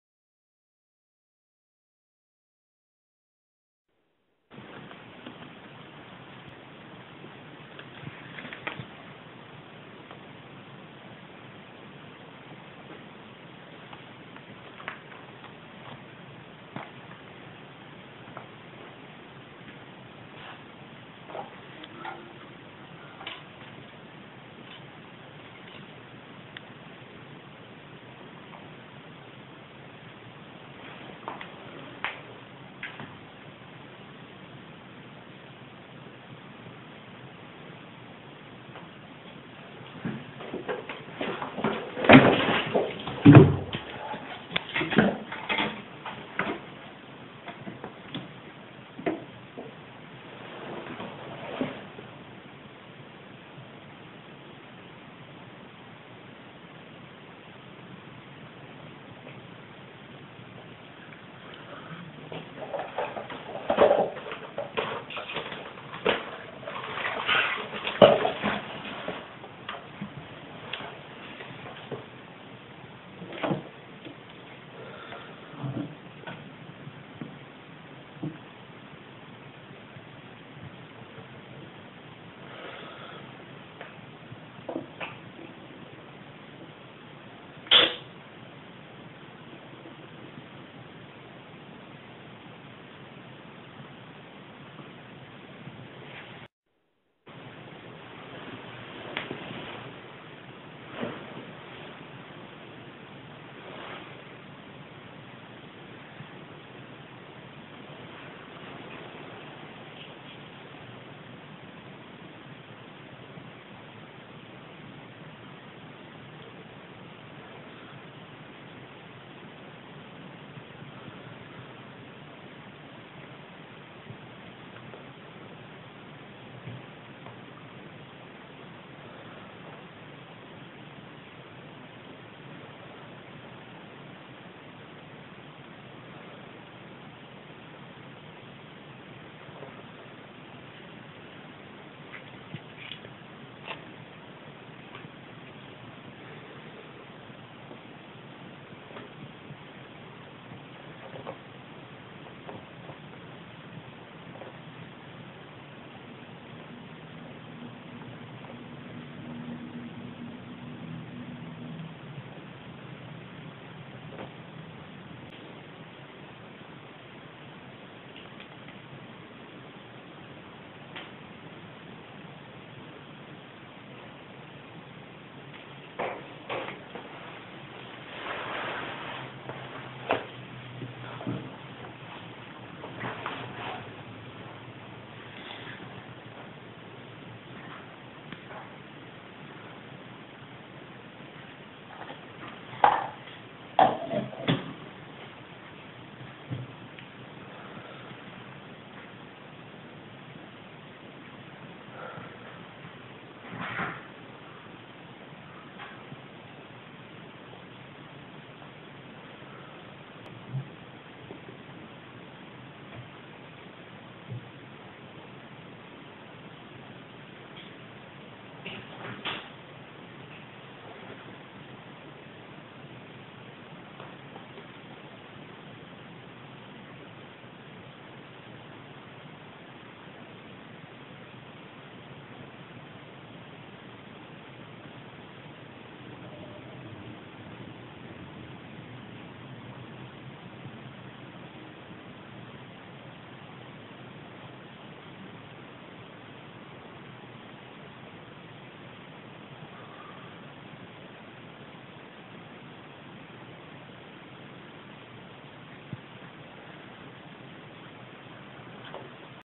pinemartin.wmv
This is a video of a Pine martin i rescued. Since i rescued him I have bn doing allot of research as well as getting professional advice. First off they are Ireland's rarest mammal!! Endangered species! It seems he/she is 8-10 weeks old. Somehow displaced from its nest. When i fond him/her, he/ was in shock and unable to walk properly. The next morning I took it to the vets and no sign of injury could be found except bruising. An anti inflammatory was given. Today is day three and I finally got it to eat some raw egg from a syringe. Also some water was drunk. I'm trying to have minimal contact with him as to make his transition back to the wild as easy for him as poss